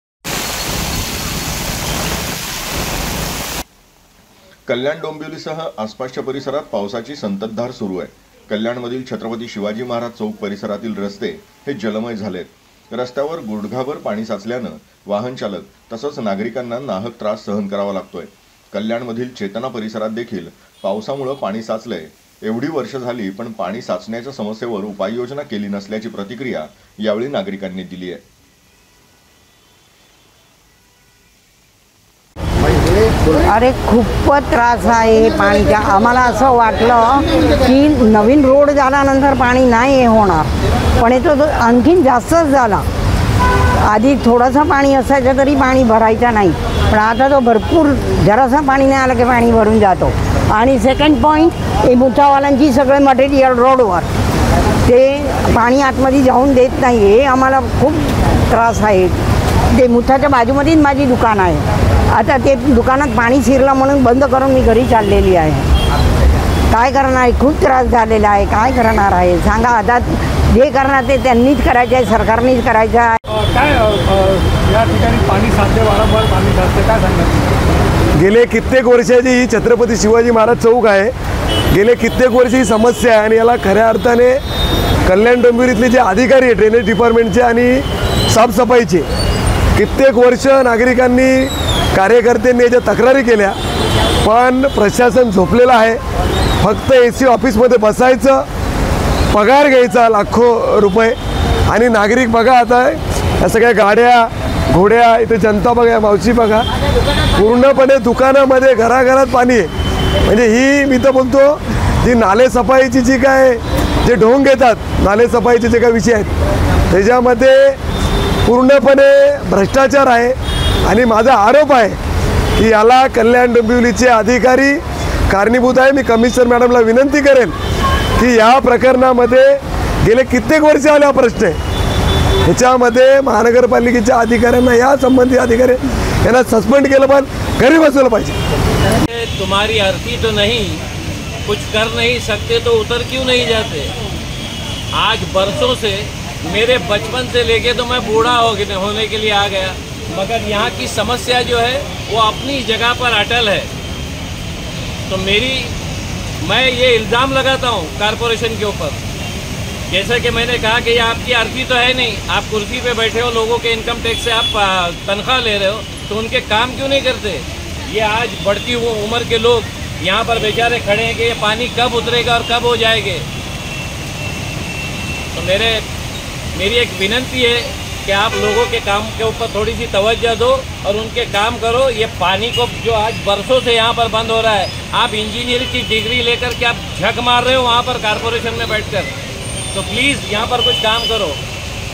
KALYAN | सतत पडणाऱ्या पावसामुळे कल्याणमधील छत्रपती शिवाजी महाराज चौक परिसरातील रस्ते जलमय...
- कल्याण-डोंबिवलीसह आसपासच्या परिसरात पावसाची संततधार सुरु आहे
- कल्याणमधील छत्रपती शिवाजी महाराज चौक परिसरातील रस्ते जलमय झाले आहे
- रस्त्यावर गुडघाभर पाणी साचल्याने वाहनचालक तसेच नागरिकांना नाहक त्रास सहन करावा लागत आहे
- कल्याणमधील चेतना परिसरात देखील पावसामुळे पाणी साचले आहे
- एवढी वर्षे झाली पण पाणी साचण्याच्या समस्येवर उपाययोजना केली नसल्याची प्रतिक्रिया नागरिकांनी दिली आहे